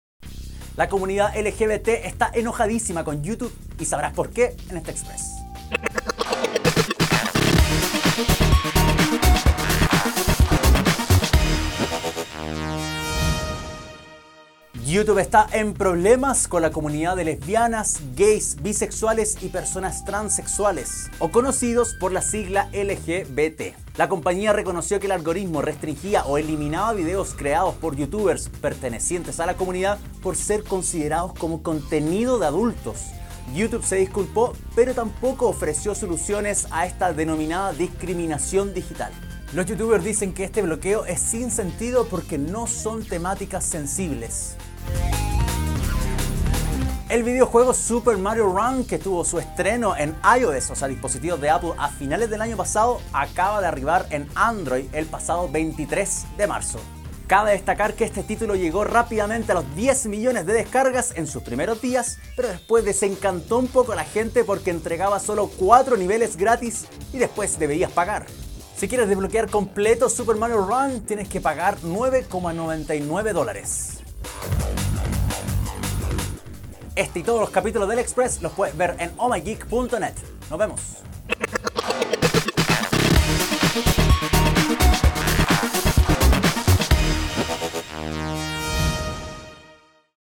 OMG! Express: YouTube vs la comunidad LGBT y Super Mario Run en Android
Enojados están los youtubers y creadores de contenidos de la comunidad LGBT con YouTube. La compañía reconoció que su algoritmo restringía o bloqueaba videos sobre contenidos que comentaban de la diversidad sexual.

Además de esa noticia, en este EXPRESS de OhMyGeek!, te contamos del arribo de Super Mario Run para Android, esperando a recibir el mismo éxito que tuvo con su debut en iOS.